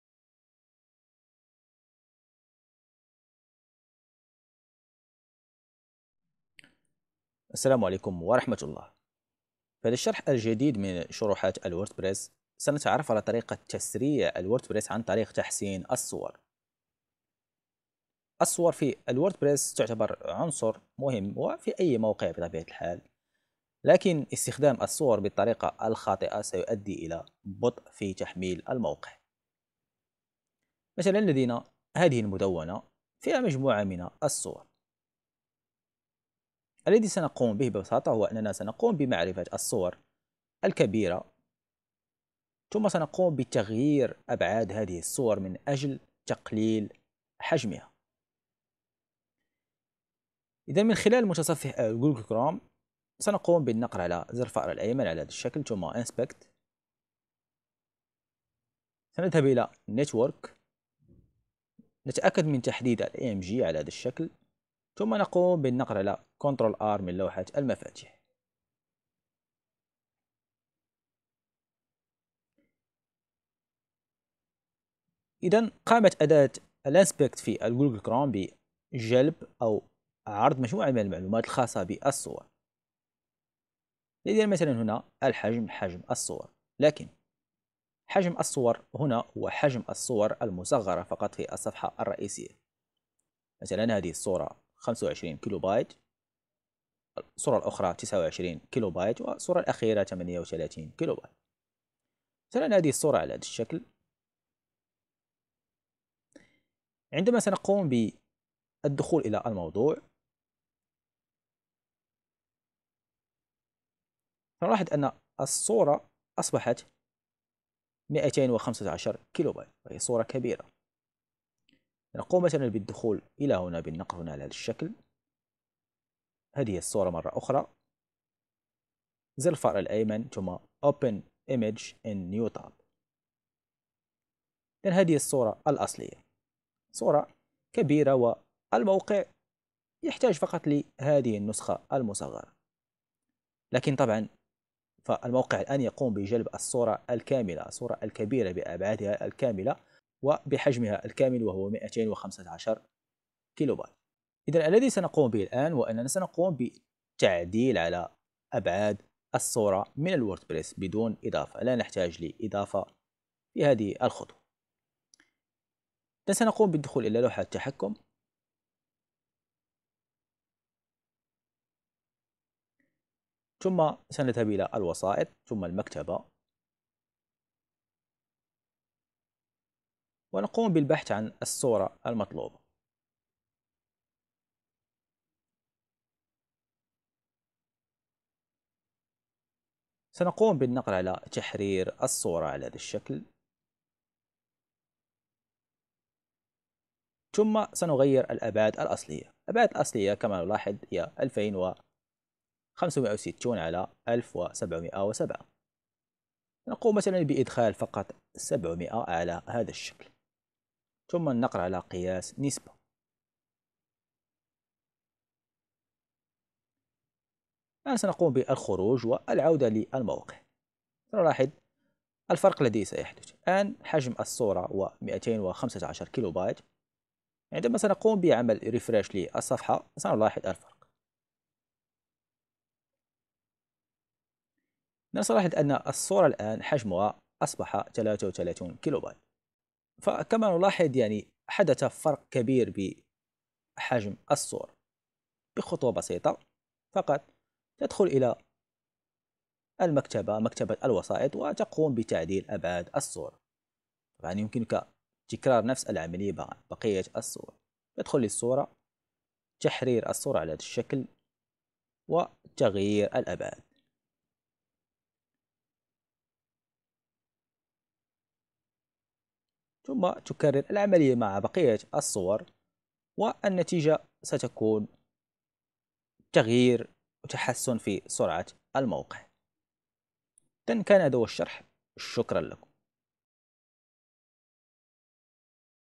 تسريع ووردبريس عن طريق تقليل حجم الصور بدون إضافة
تعلم كيف تقوم بتقليل وضغط حجم الصور في ووردبريس بدون إضافة من أجل تسريع تحميل الموقع

أيضا يمكنك متابعتنا على مواقع التواصل الإجتماعي فيسبوك و انستغرام وتيك توك و تويتر على